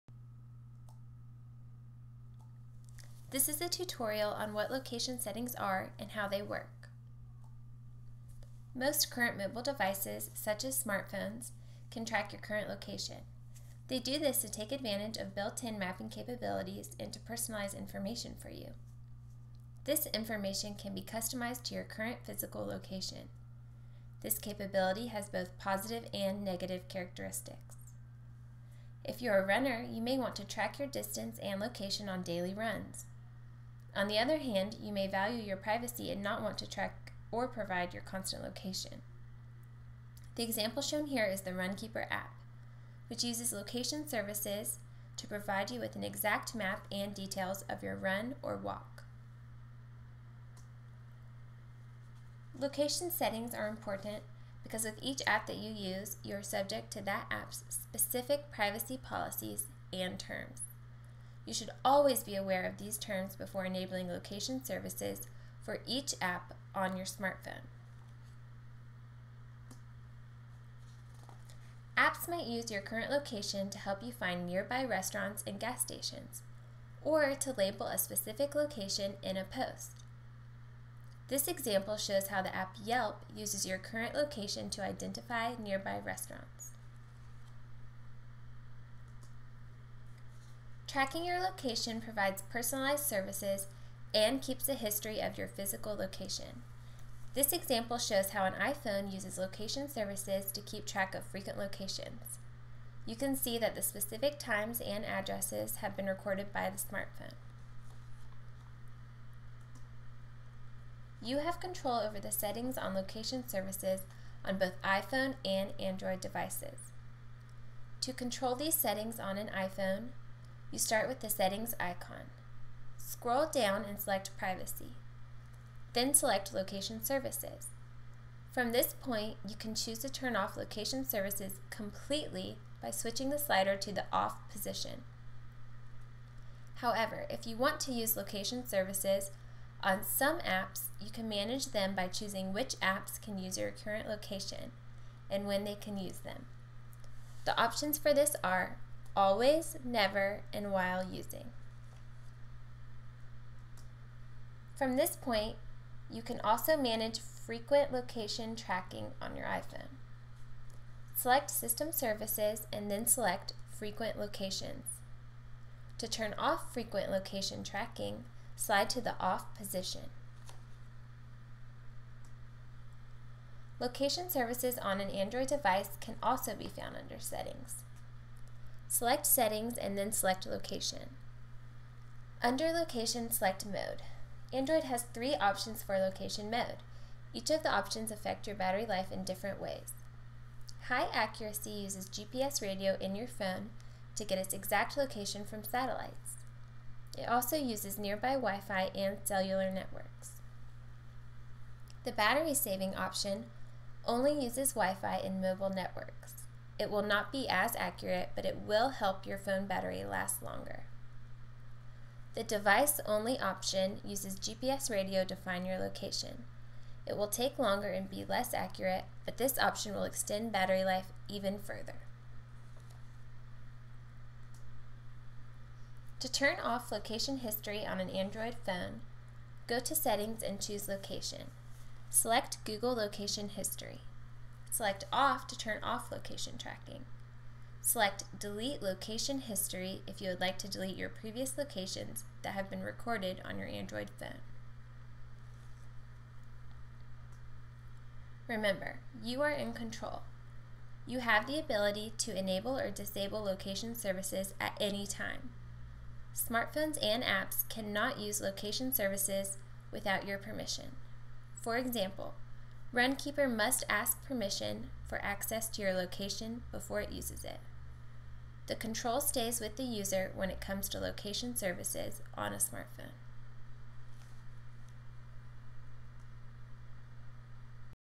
How to Use Location Settings on a Smartphone
Location settings enable you to control whether and how your phone tracks your location. This video tutorial shows you how to use and control location settings on an iPhone and on an Android device.